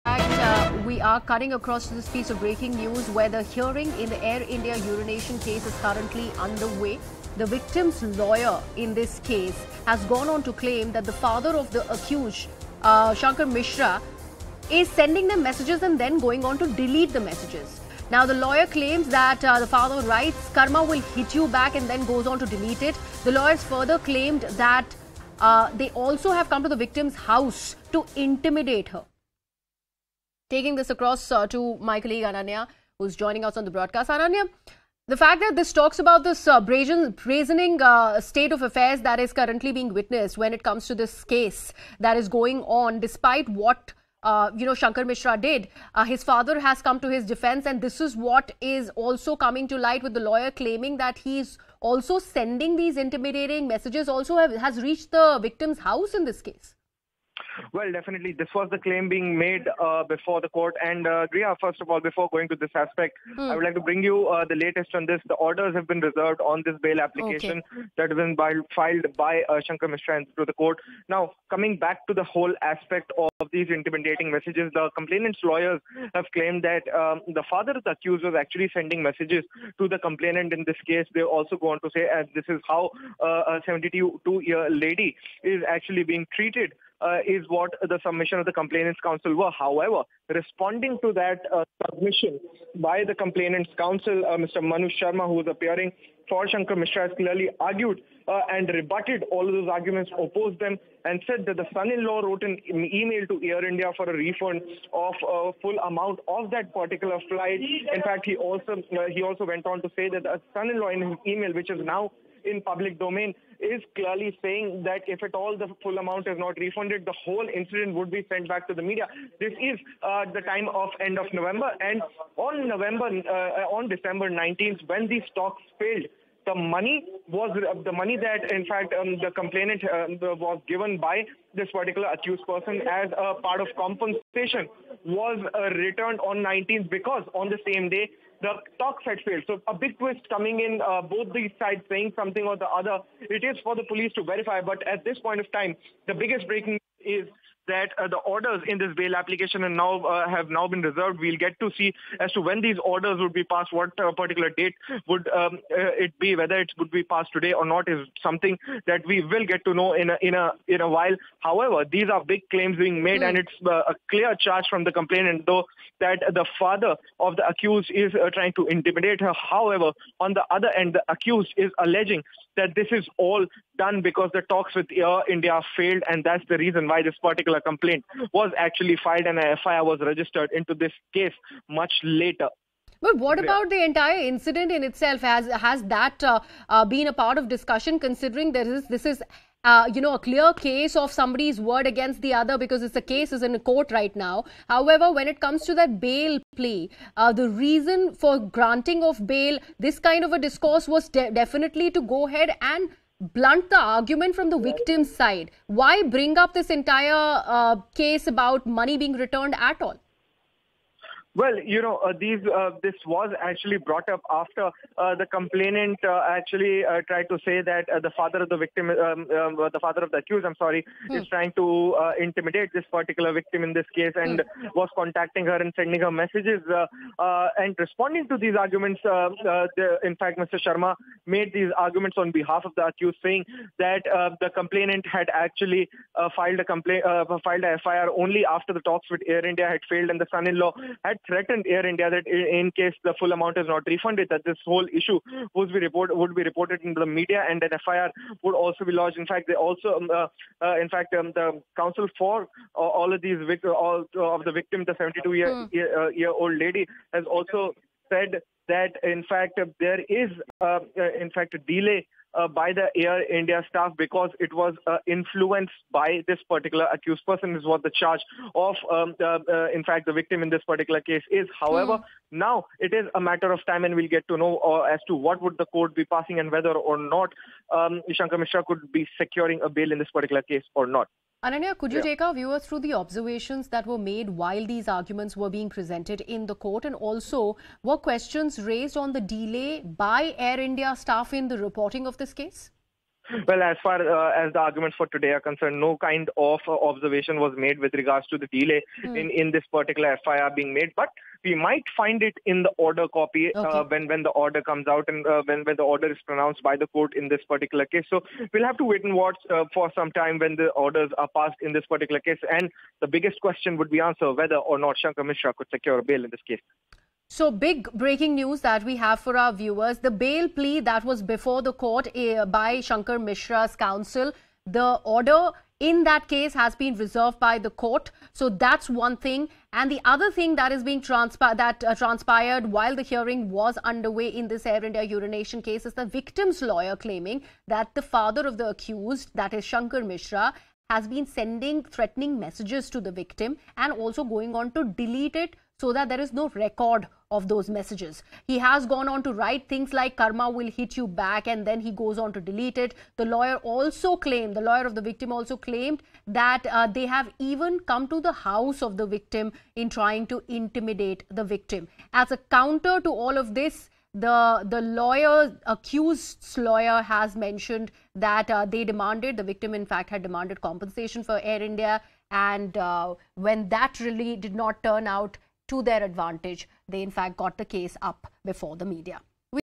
Air India Urination Case News Update | Bail Plea Order Reserved | Latest English News | News18
Breaking News | Air India Urination Case: Bail Plea Order Reserved.

"The father of the accused is sending me messages and is deleting it. He writes 'Karma will hit you back' and then deletes it": Complainant's lawyer

n18oc_india